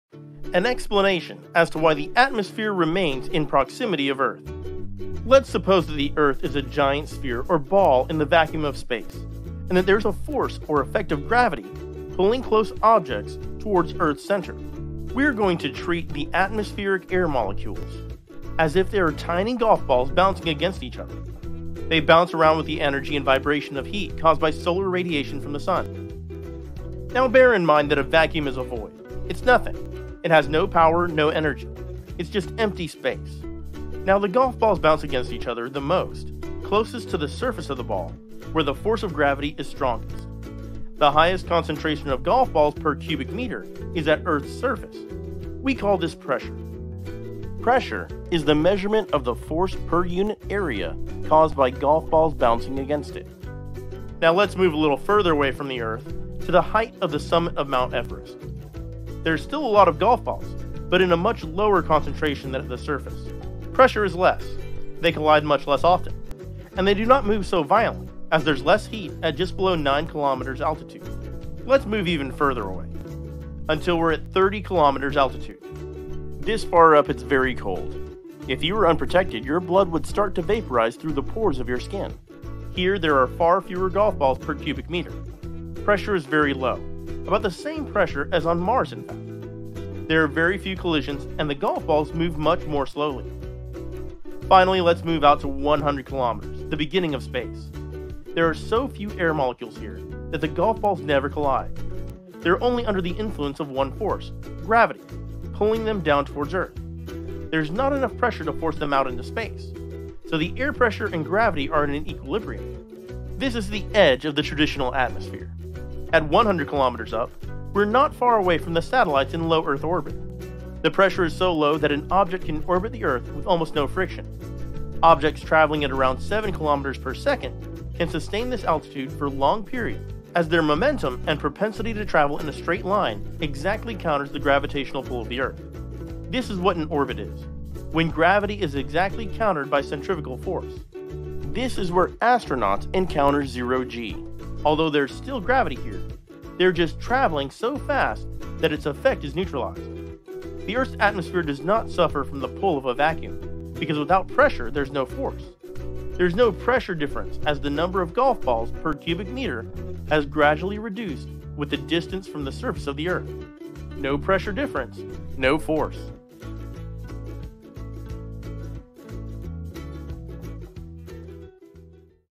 Gravity and Earth's Atmosphere
Why the atmosphere decreases in pressure with altitude and what it means.